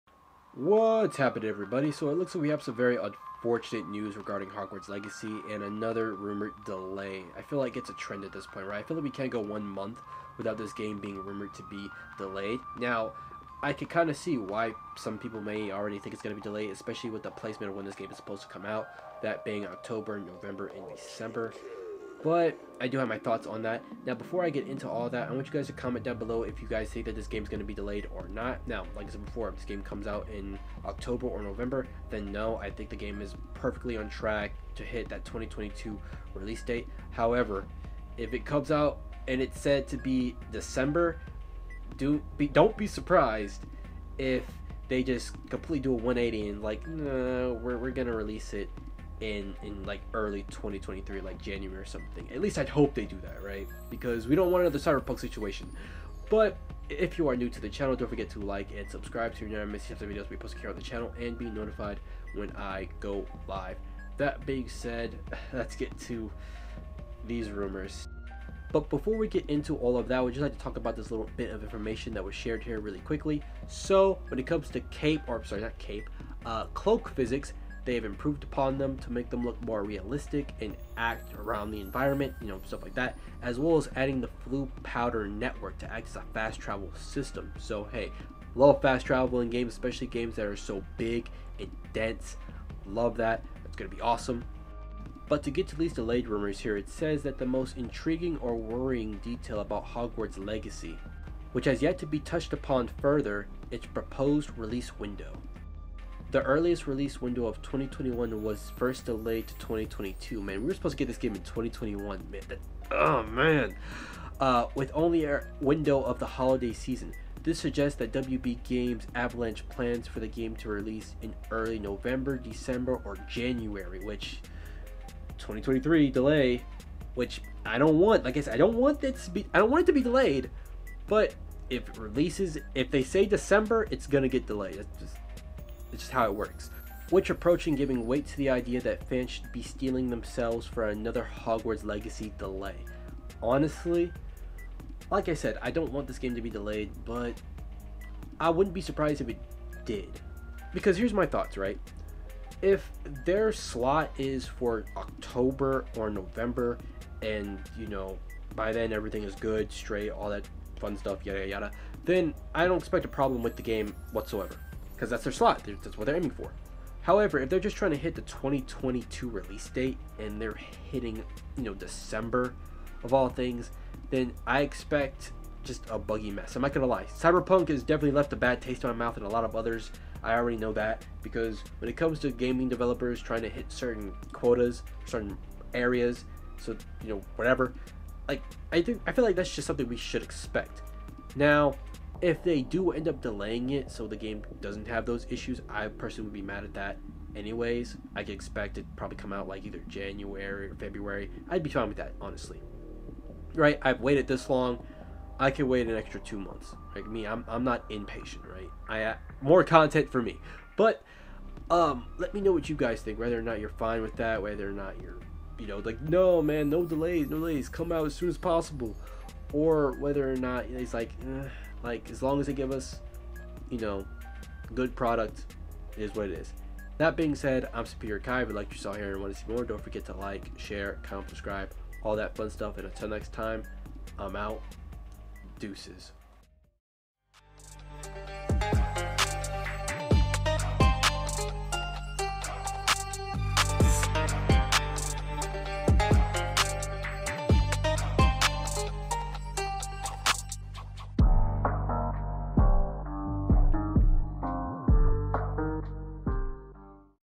Hogwart Legacy Delay rumors
More rumors about Hogwarts Legacy being delayed again are popping up also it's been Confirmed that floo powder will be used as Fast travel